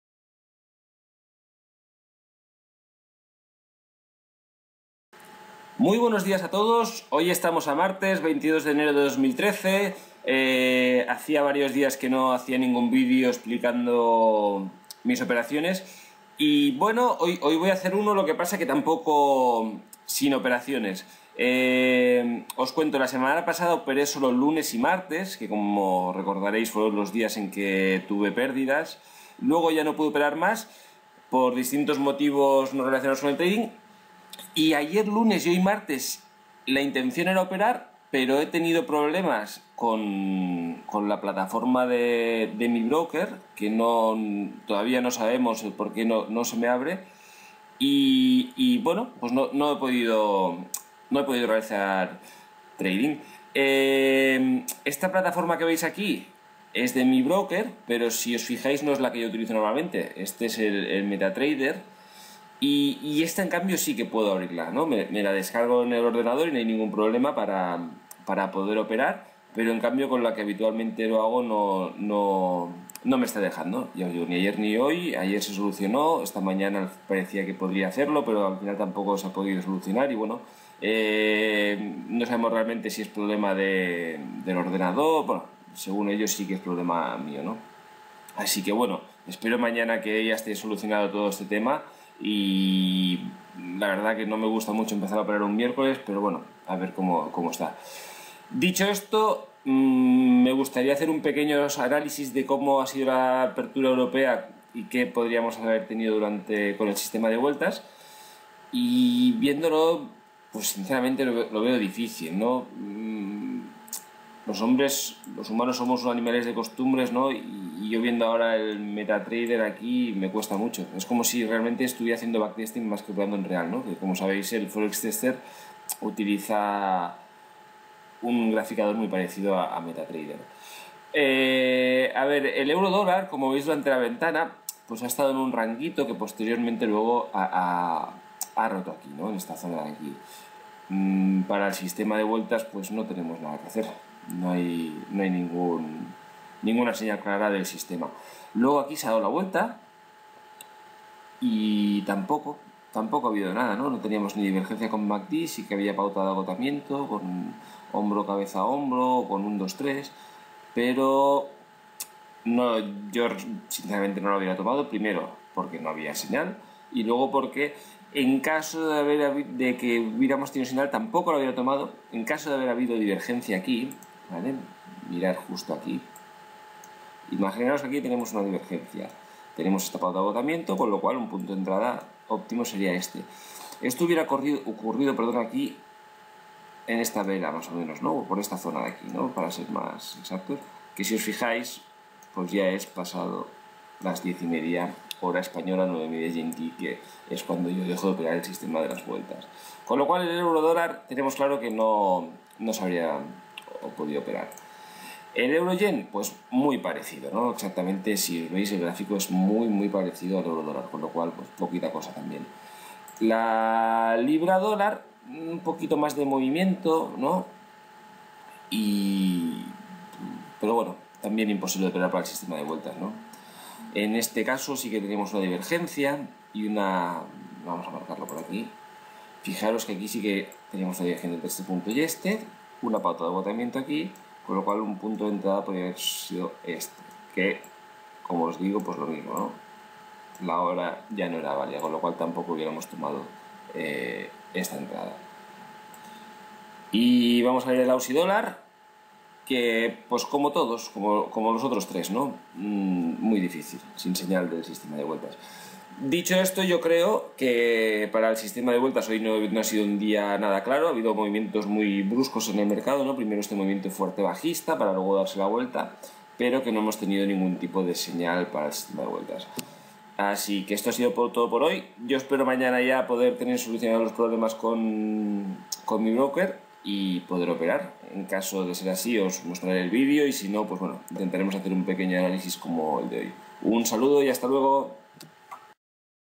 Trading en Forex
Varios días sin poder operar en el mercado, la semana pasada solo dos por varios motivos y esta todavía ninguno por problemas la plataforma de trading, espero mañana poder hacerlo aunque de hacerlo lo haré con poco riesgo ya que estaremos a miércoles y como sabéis mi principal objetivo es acabar la semana en BE.

Un saludo y feliz trading